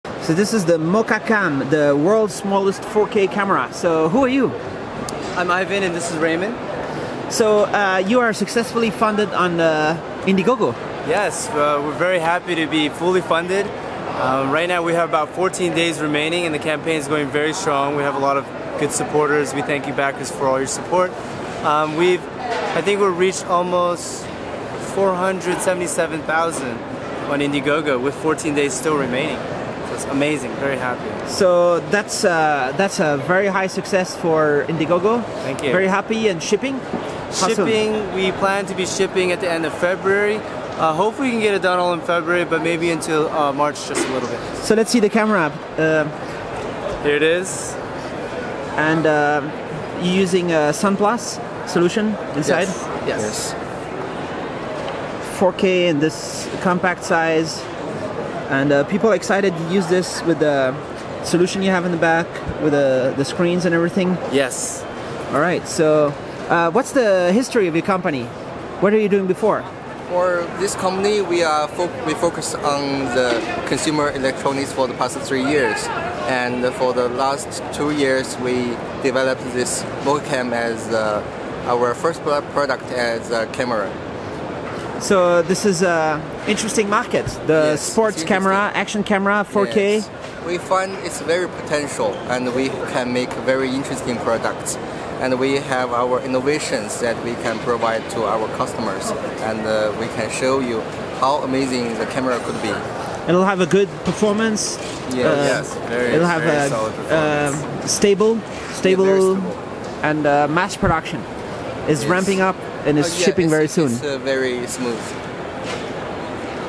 Mokacam World's Smallest 4K Camera – Funded on IndieGoGo
Mokacam is the World’s Smallest 4K Action Camera weighing just 79g. Featuring a 16 mega pixel sensor, 152 degree wide lens, and support up to 4K H.264 video recording. Accessories include a rotating screen which doubles as a view finder, and a magnetic snap-on battery. Early bird pricing for Mokacam starts at just $99 on their IndieGoGo campaign
Mokacam is nearly 1000% funded through IndieGoGo.